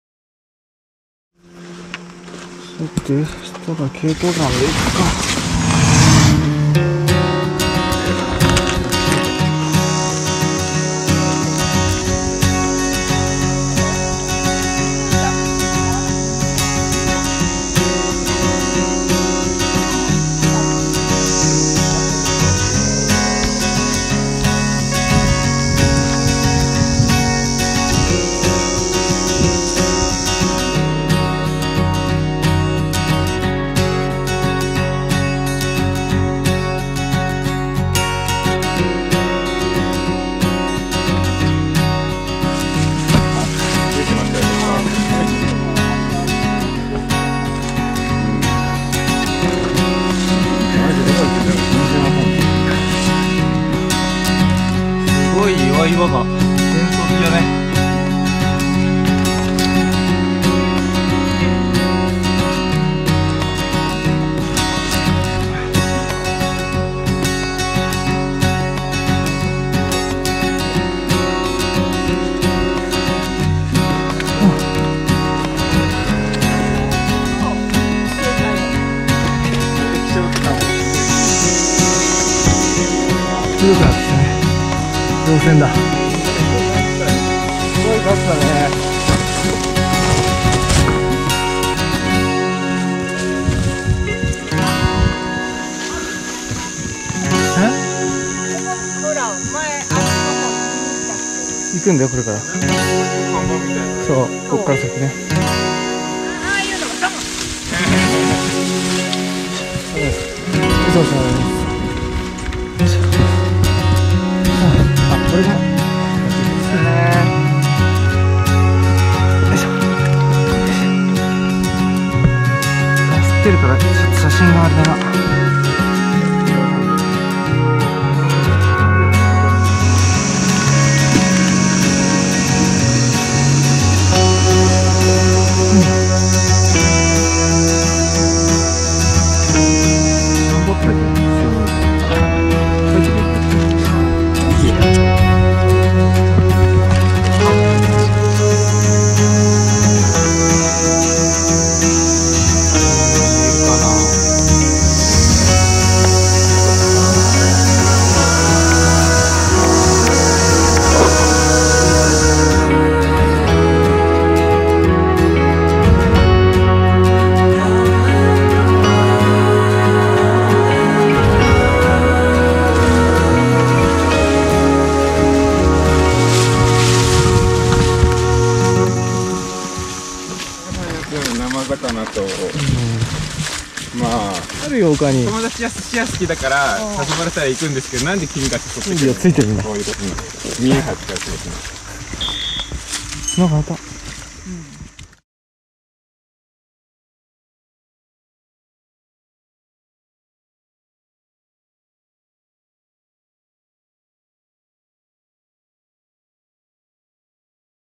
2024/08/18 東ヌプカウシヌプリとナキウサギ
ちょっと足を痛めたため、大雪山系の南側の手軽な場所へ。ナキウサギの撮影で人気のスポットへ登りました。